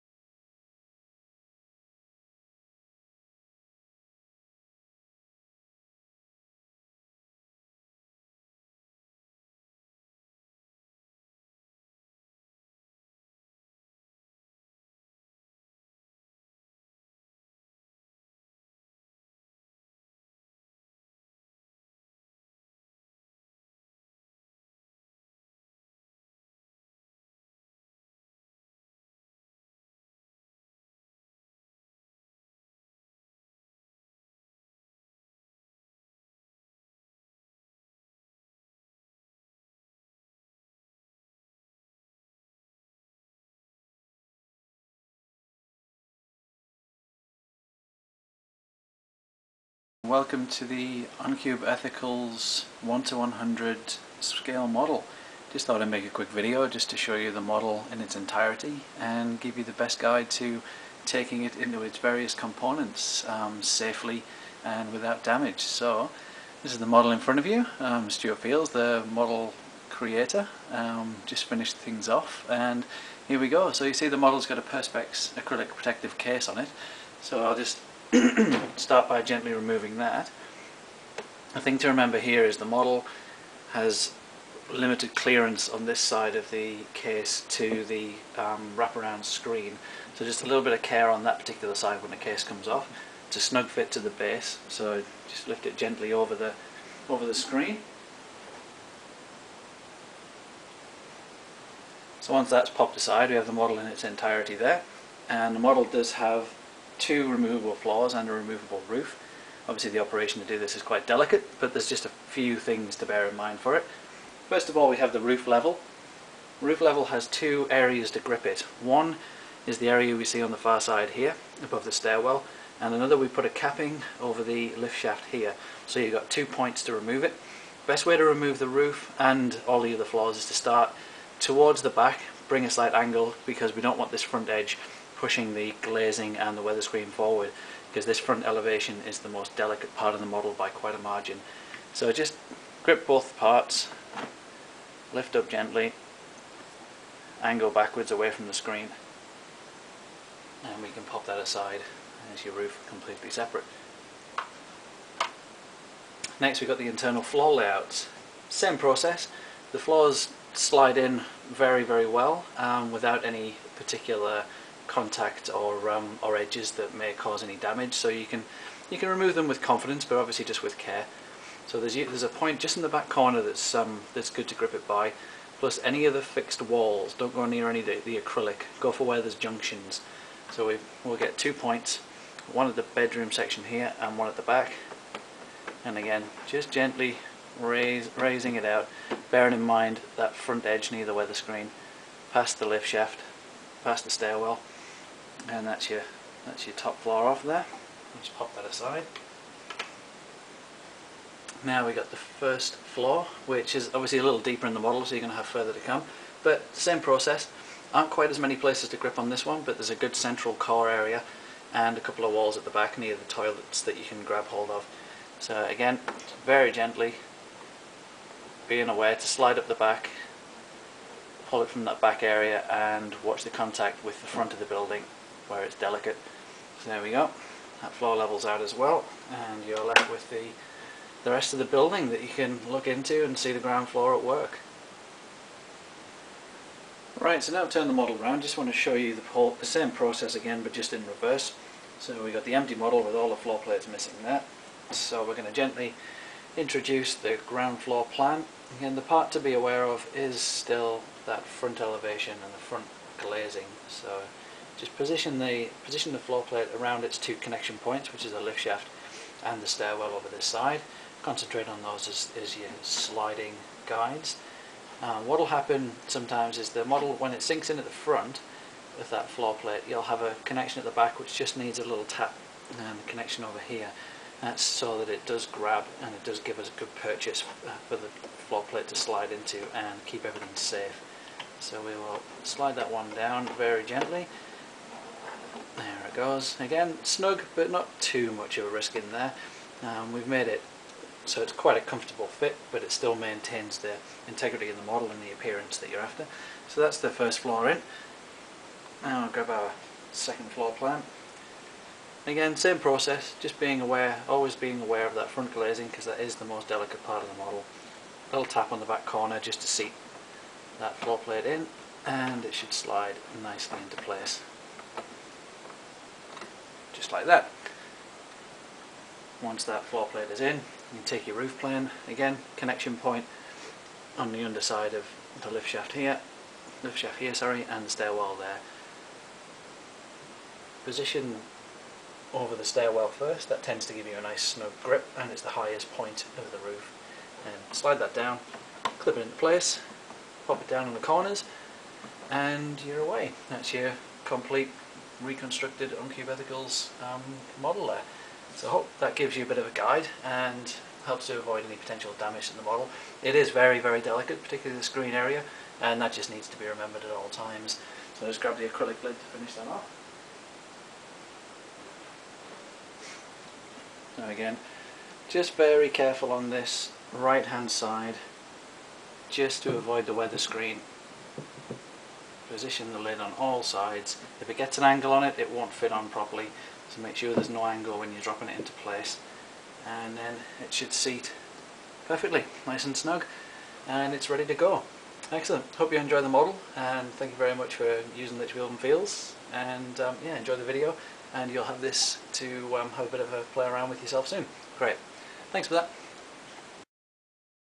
Encube Ethicals model.wmv
Instructional video of how to handle the Encube Ethicals model created by Litchfield + Fields Ltd